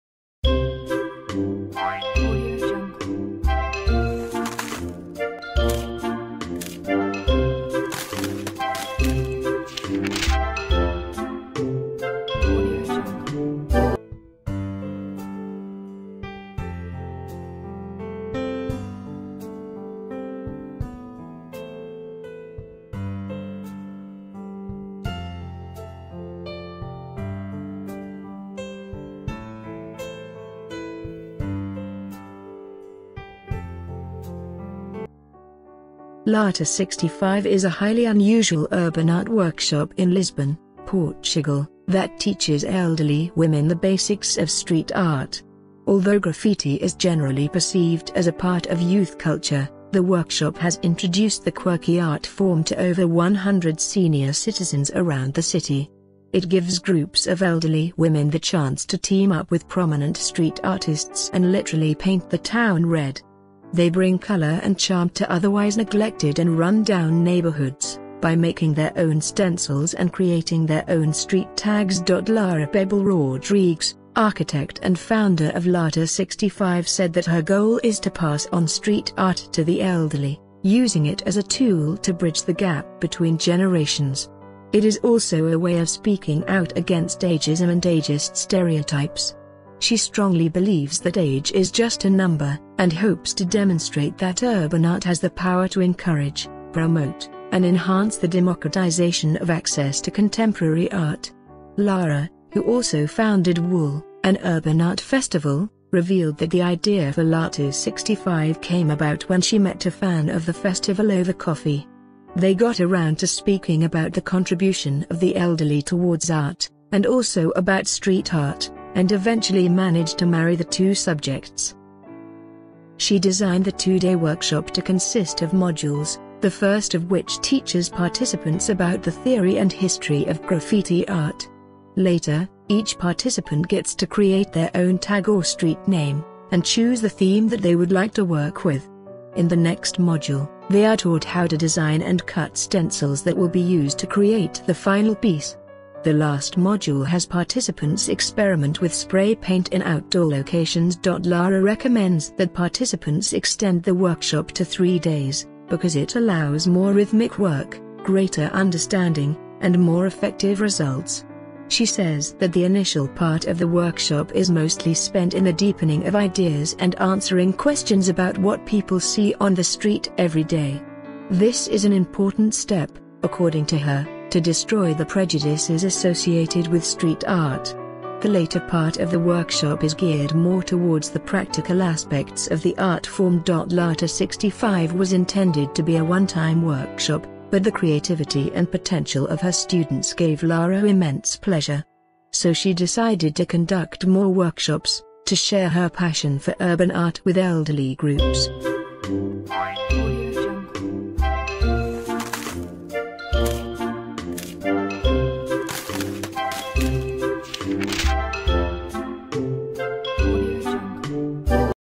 Never too Old for Art – Portugal’s Granny Graffiti Gang Lata
Lata 65 is a highly unusual urban art workshop in Lisbon, Portugal, that teaches elderly women the basics of street art. Although graffiti is generally perceived as a part of youth culture, the workshop has introduced the quirky art form to over 100 senior citizens around the city. It gives groups of elderly women the chance to team up with prominent street artists and literally paint the town red. They bring color and charm to otherwise neglected and run-down neighborhoods, by making their own stencils and creating their own street tags.

Lara Pebble Rodrigues, architect and founder of Lata 65 said that her goal is to pass on street art to the elderly, using it as a tool to bridge the gap between generations. It is also a way of speaking out against ageism and ageist stereotypes. She strongly believes that “age is just a number,” and hopes to demonstrate that urban art has the “power to encourage, promote, and enhance the democratisation of access to contemporary art.”

Lara, who also founded ‘WOOL’, an urban art festival, revealed that the idea for Lata 65 came about when she met a fan of the festival over coffee. They got around to speaking about the contribution of the elderly towards art, and also about street art, and eventually managed to marry the two subjects.

She designed the two-day workshop to consist of modules, the first of which teaches participants about the theory and history of graffiti art. Later, each participant gets to create their own ‘tag’ or street name, and choose the theme that they would like to work with. In the next module, they are taught how to design and cut stencils that will be used to create the final piece. The last module has participants experiment with spray paint in outdoor locations.

Lara recommends that participants extend the workshop to three days, because it “allows more rhythmic work, greater understanding, and more effective results.” She says that the initial part of the workshop is mostly spent in the deepening of ideas and answering questions about what people see on the street every day. This is an important step, according to her, to destroy the prejudices associated with street art. The later part of the workshop is geared more towards the practical aspects of the art form.

granny-graffiti-gang2

Lata 65 was intended to be a one-time workshop, but the creativity and potential of her students gave Lara “immense pleasure.” So she decided to conduct more workshops, to share her passion for urban art with elderly groups.